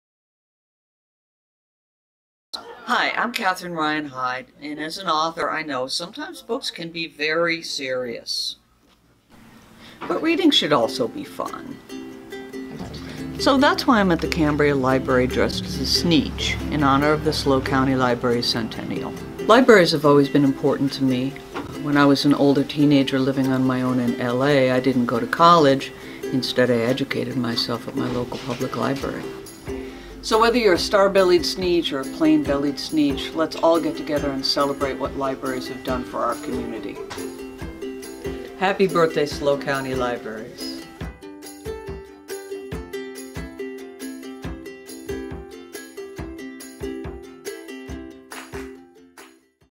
A Serious Message from Catherine Ryan Hyde
Bestselling author Catherine Ryan Hyde has a serious message in honor of the San Luis Obispo County Library's Centennial. Okay... maybe it's not that serious, but she still has some important things to say. Like did you know that libraries changed her life as a teen? And that she's a big fan of Dr. Seuss?

Catherine is a prolific author of 36 books with her novel Stay set to be released December 3.

A big thank you to Makeup By Philly and to Pacific Hair.